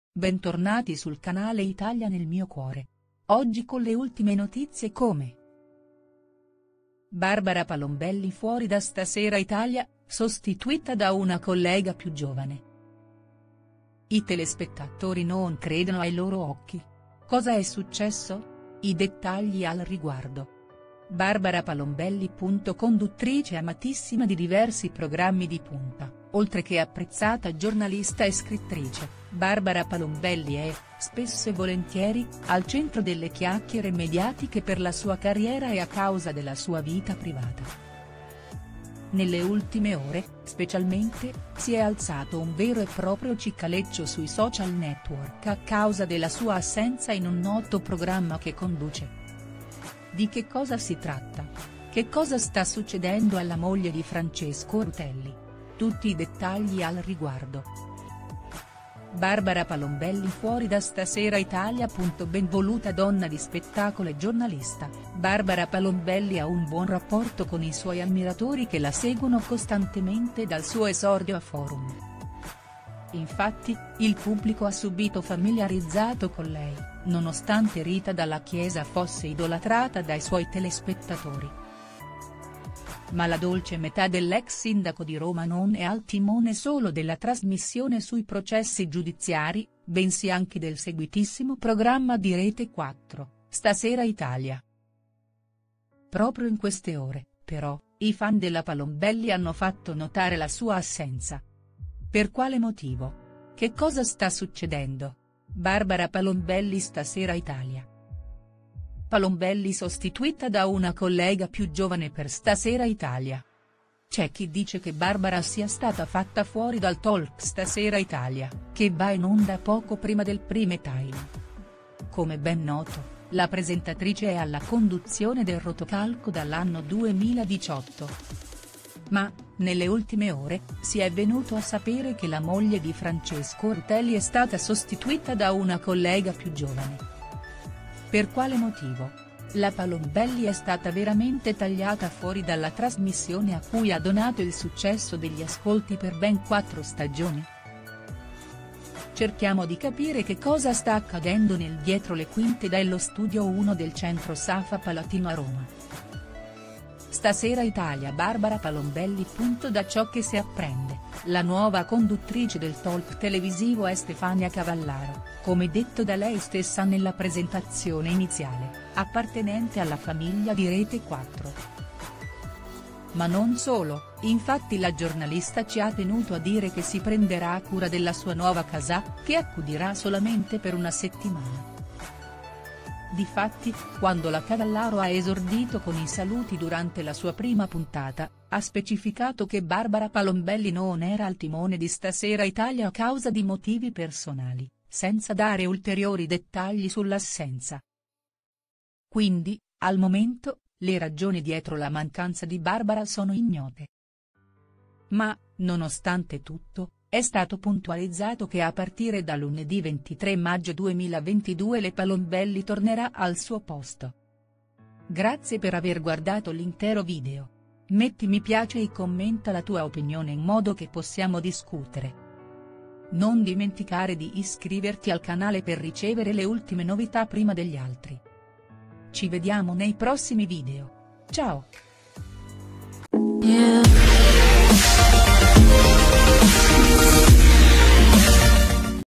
Barbara Palombelli fuori da Stasera Italia sostituita da una collega più giovane
Conduttrice amatissima di diversi programmi di punta, oltre che apprezzata giornalista e scrittrice, Barbara Palombelli è, spesso e volentieri, al centro delle chiacchiere mediatiche per la sua carriera e a causa della sua vita privata.

Nelle ultime ore, specialmente, si è alzato un vero e proprio cicaleccio sui social network a causa della sua assenza in un noto programma che conduce. Di che cosa si tratta? Che cosa sta succedendo alla moglie di Francesco Rutelli? Tutti i dettagli al riguardo.